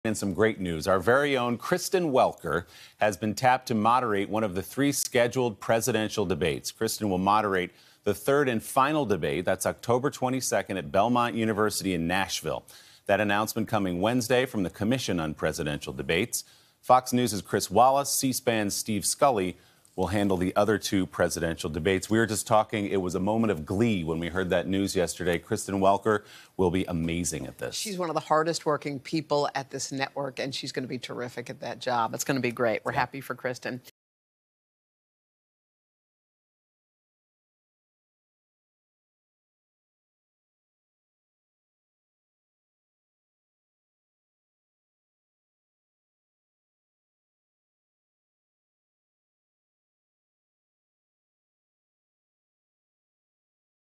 Moderators Set For Presidential Debates: Kristen Welker, Chris Wallace, Steve Scully | TODAY
NBC News’ Kristen Welker has been tapped to moderate the third and final presidential debate Oct. 22 at Belmont University in Nashville. Fox News’ Chris Wallace and C-SPAN’s Steve Scully will handle the other two debates.